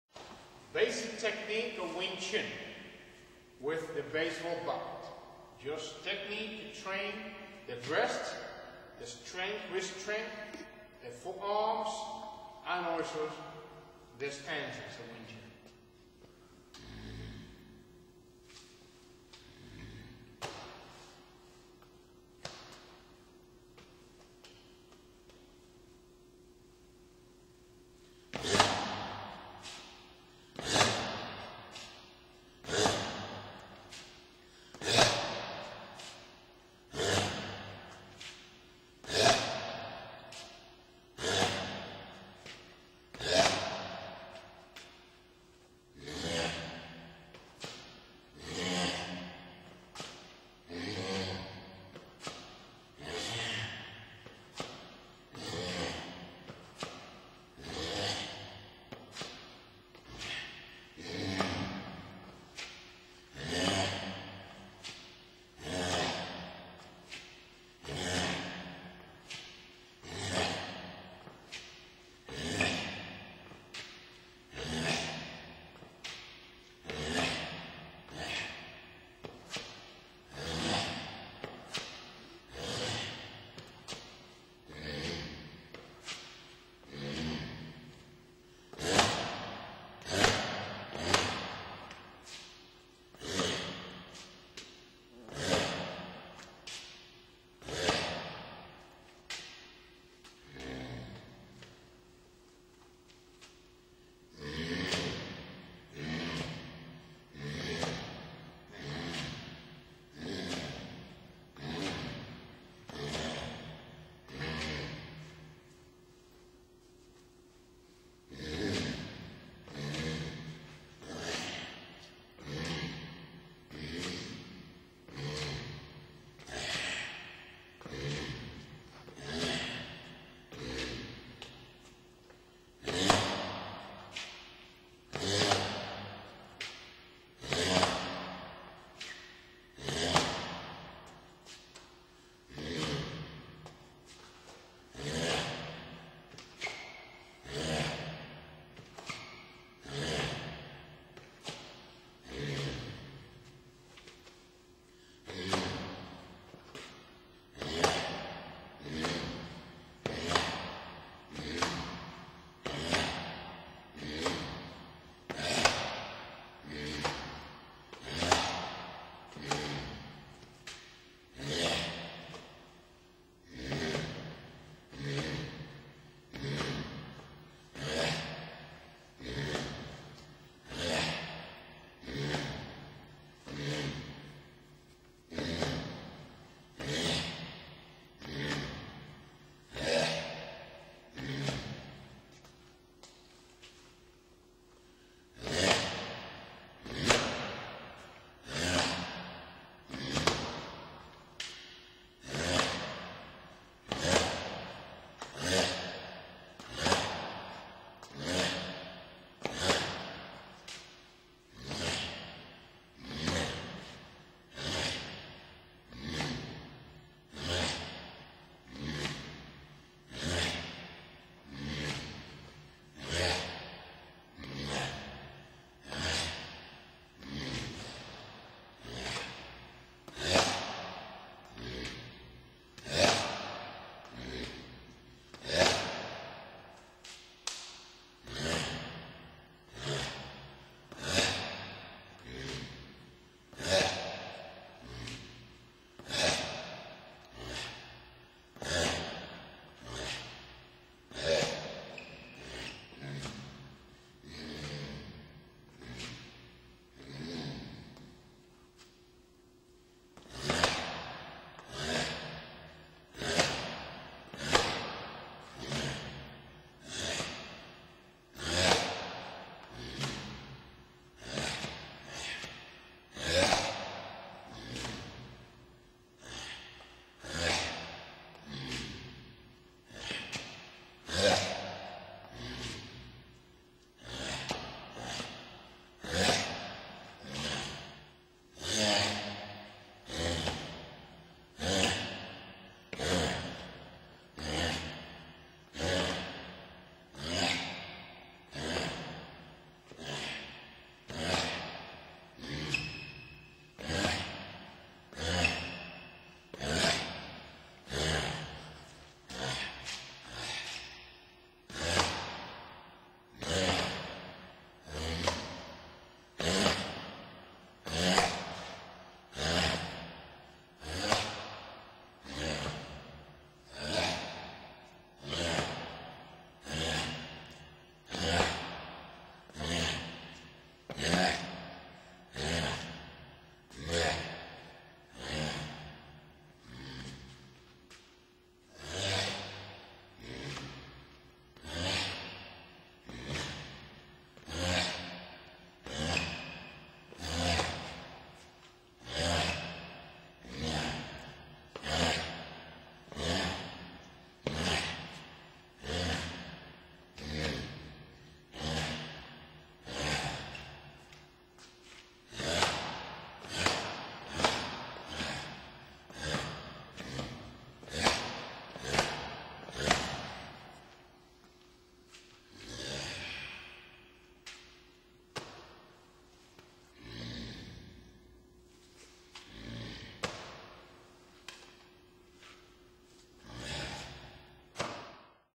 Wing Chun Baseball Bat Exercises
If you are interested on improving your chinese boxing stance but above all your striking power then you should really concentrate on understanding what Calasanz is doing here, when he is using a simple tool and that is a baseball bat.